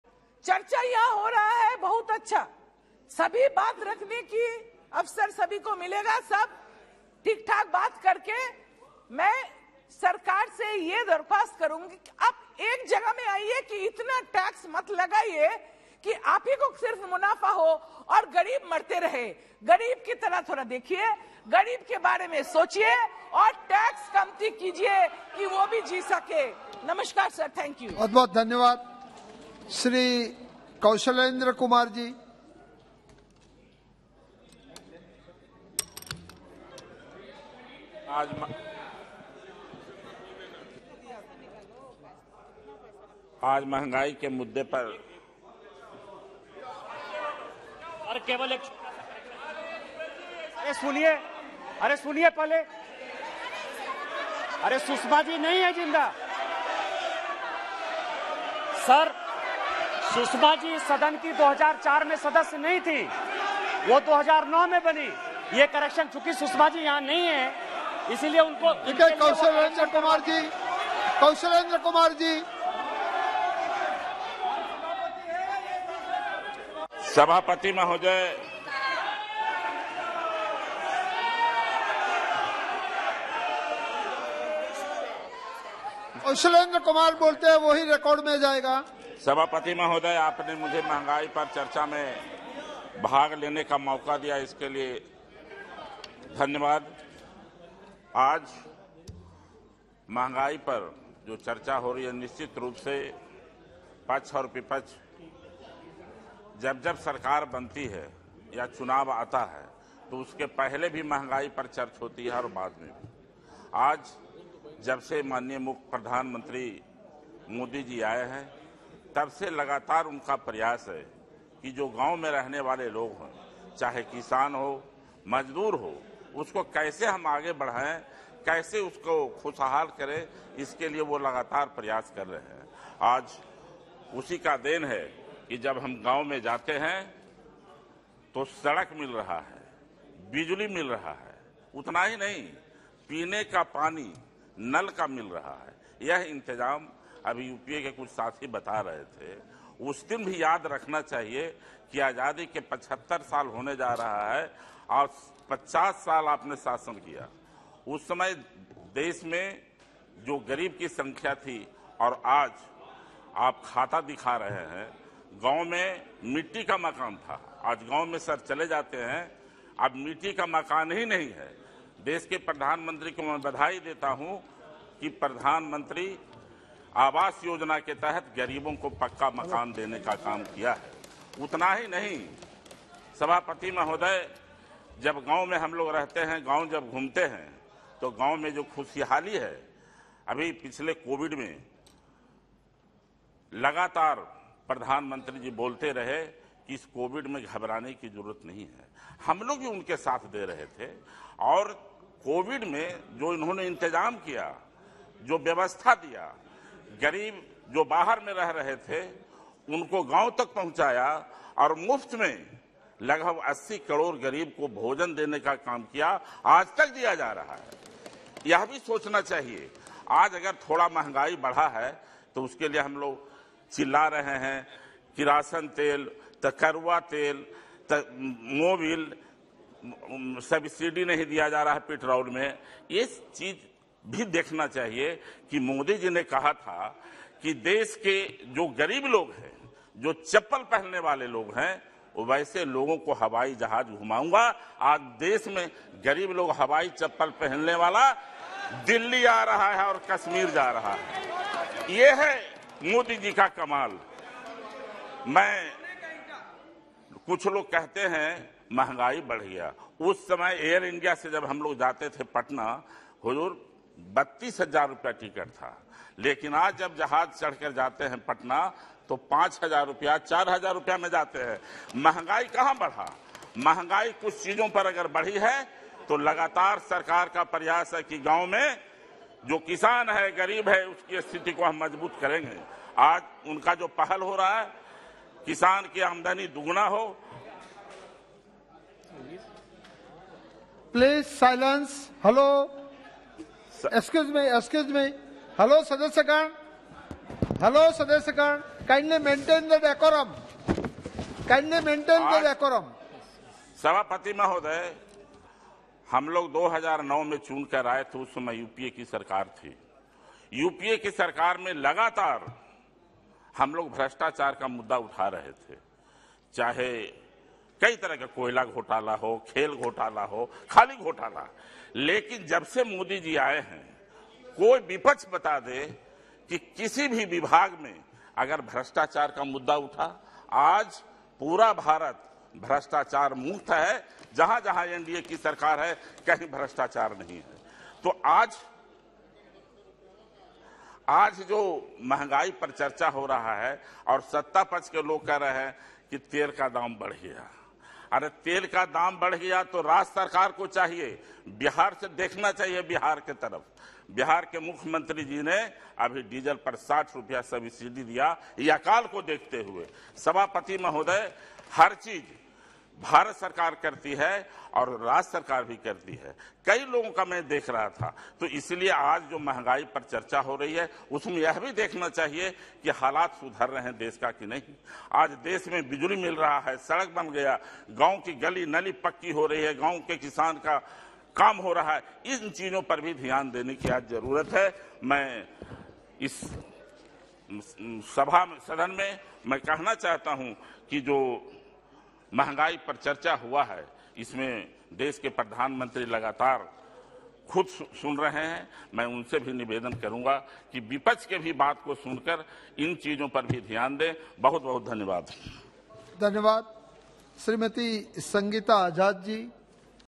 Kaushalendra Kumar | Discussion under Rule 193 on price rise
Sansad TV